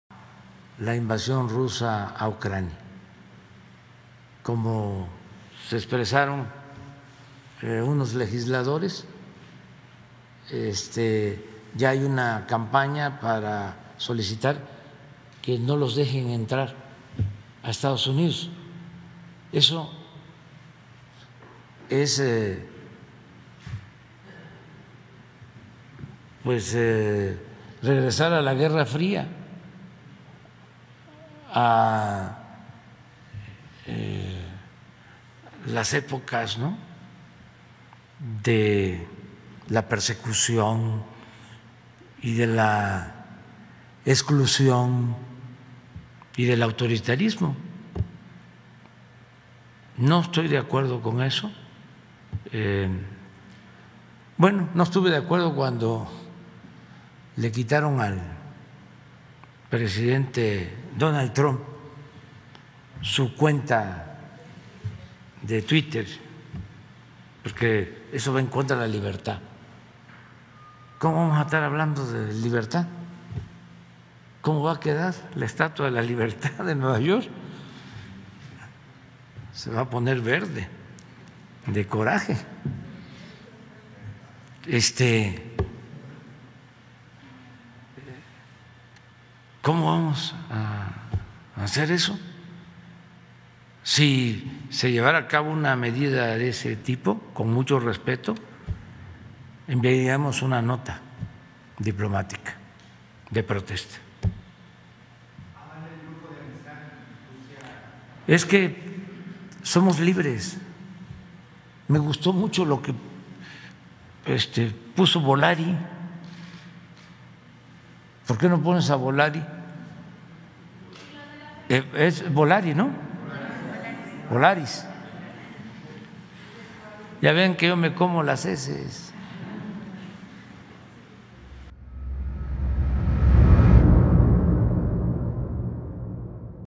AMLO señala que si cancelan visas a legisladores, se enviará carta a EEUU
Video El presidente AMLO destacó que no se puede censurar el pensamiento de legisladores sobre el tema Rusia y que estará al pendiente si avanza la idea de retirar visas.